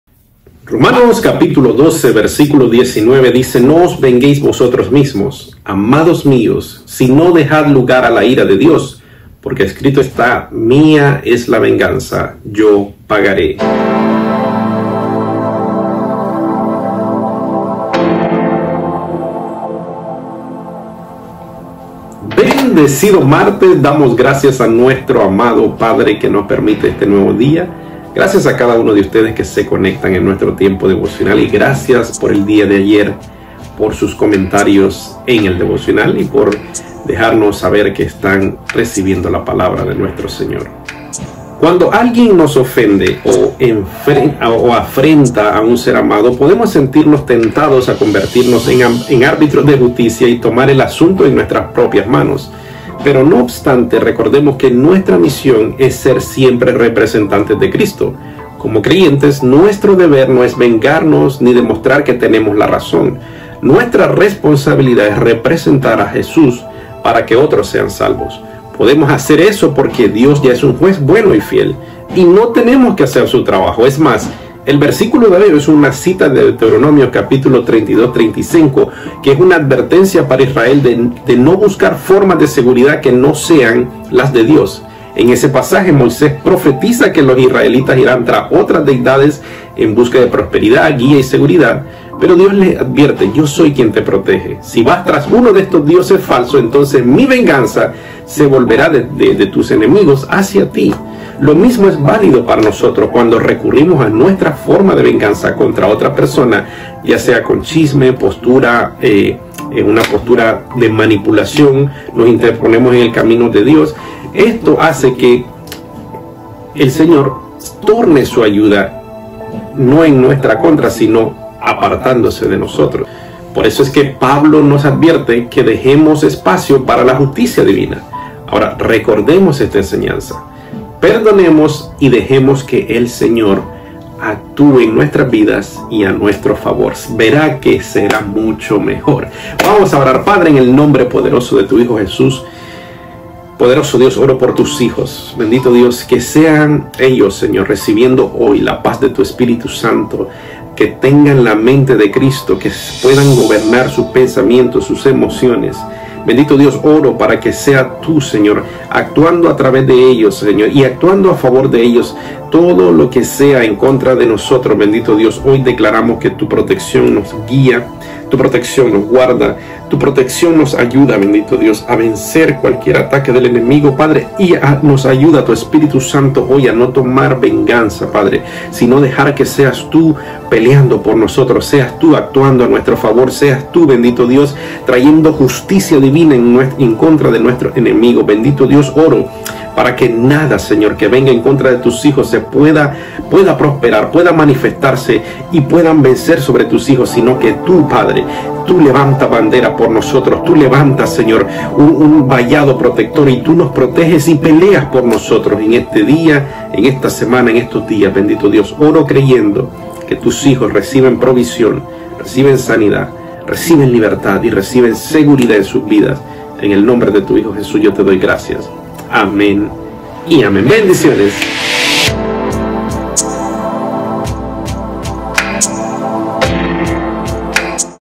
Vida con proposito Octubre 25
Bendecido Martes, Recuerda que tu Padre Eterno pelea por ti no hagas justicia a tu manera, mejor espera y veras el poderoso gigante pelear por ti Follow us on Facebook, Instagram, twitter, snapchat, or visit us at 1306 W 49 street Hialeah Fl 33012.